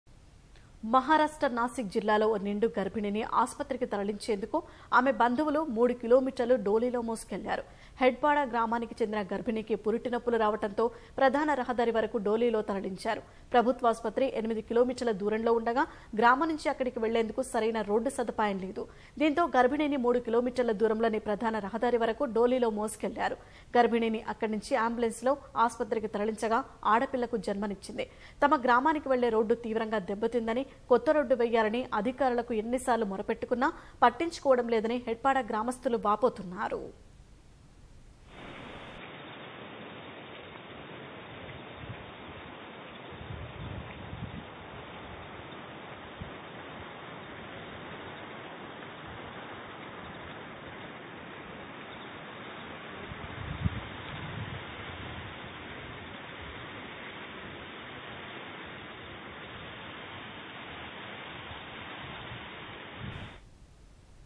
Pregnant Woman in Nasik Carried on Doli for 3 km | Maharashtra | డోలిలో నిండు గర్భిణి తరలింపు
మహారాష్ట్ర నాసిక్ జిల్లాలో. ఓ నిండు గర్భిణిని ఆస్పత్రికి తరలించేందుకు
ఆమె బంధువులు 3కిలోమీటర్లు డోలిలో మోసుకెళ్లారు. హెడ్ పాడ గ్రామానికి చెందిన గర్భిణికి పురిటినొప్పులు రావటంతో. ప్రధాన రహదారి వరకు డోలీలో తరలించారు.
ప్రభుత్వాసుపత్రి 8 కిలోమీటర్ల దూరంలో ఉండగా. గ్రామం నుంచి అక్కడికి వెళ్లేందుకు సరైన రోడ్డు సదుపాయం లేదు. దీంతో గర్భిణిని 3కిలోమీటర్ల దూరంలోని... ప్రధాన రహదారి వరకూ డోలీలో మోసుకెళ్లారు. గర్భిణిని అక్కడి నుంచి అంబులెన్స్ లో ఆస్పత్రికి తరలించగా. ఆడపిల్లకు జన్మనిచ్చింది. తమ గ్రామానికి వెళ్లే రోడ్డు తీవ్రంగా దెబ్బతిందని. కొత్త రోడ్డు వేయాలని అధికారులను ఎన్నిసార్లు మెురపెట్టుకున్నా పట్టించుకోవడం లేదని హెడ్ పాడ గ్రామస్తులు వాపోతున్నారు.